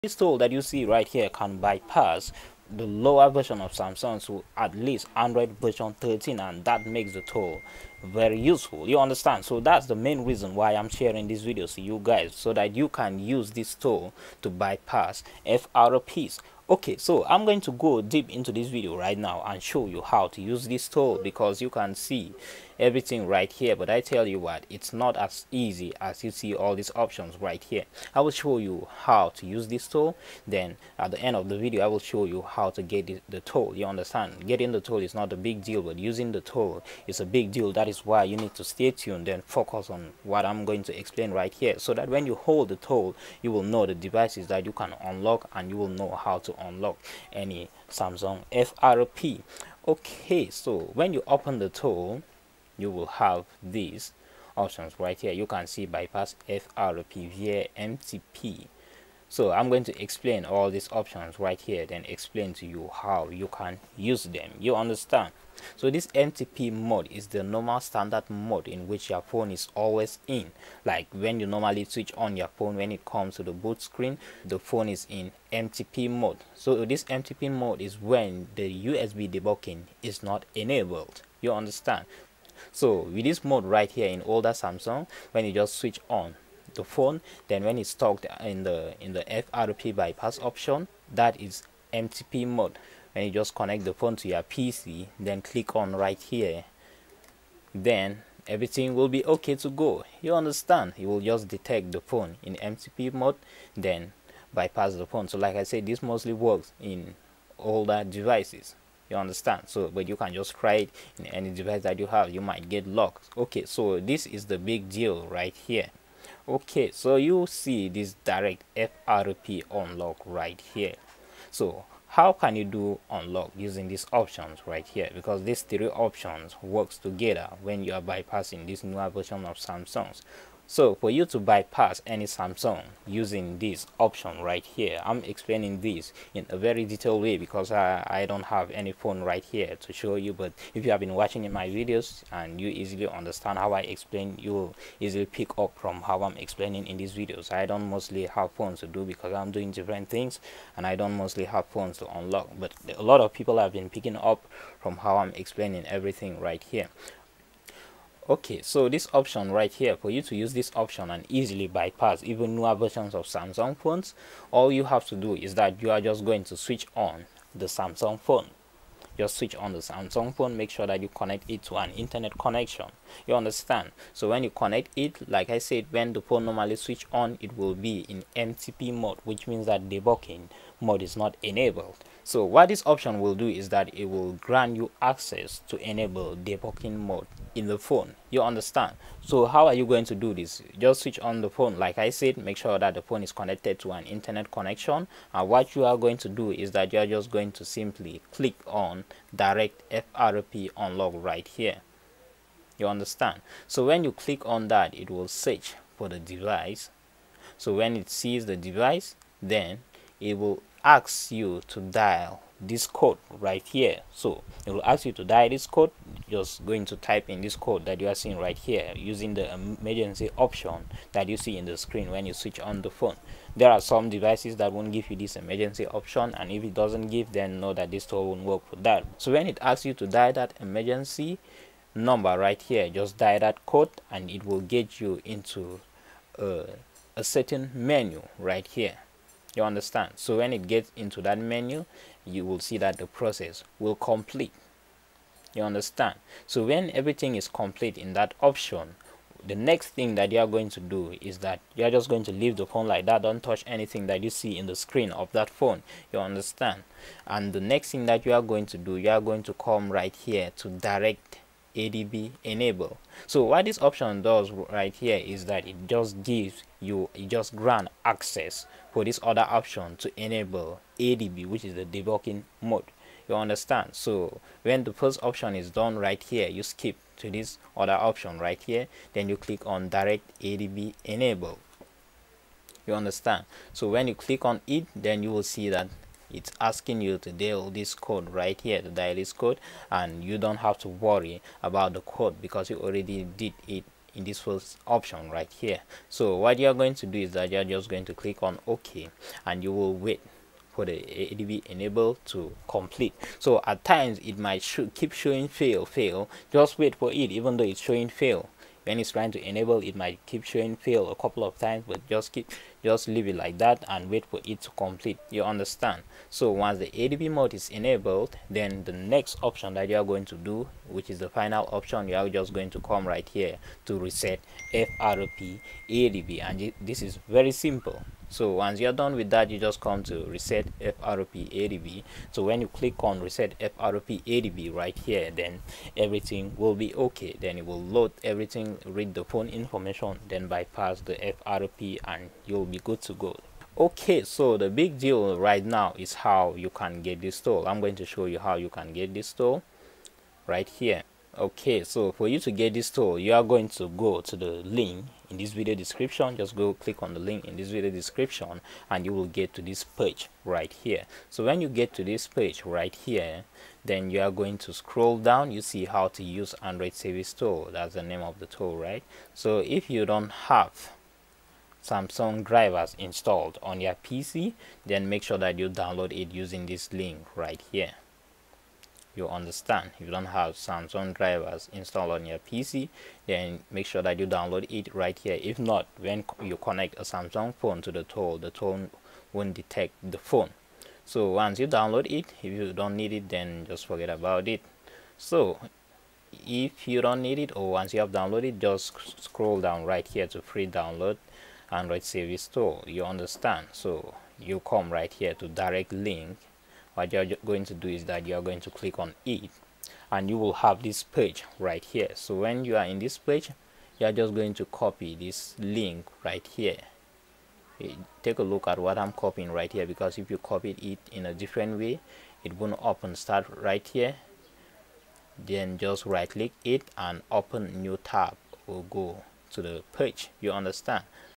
Free Samsung FRP UnlockTool
The frp unlock tool that i am sharing in this video will help you in unlocking any samsung frp lock.

Do not Try This Method on Stolen Phones, This is a serious Crime, If You are Trying to Unlock a Stolen Phone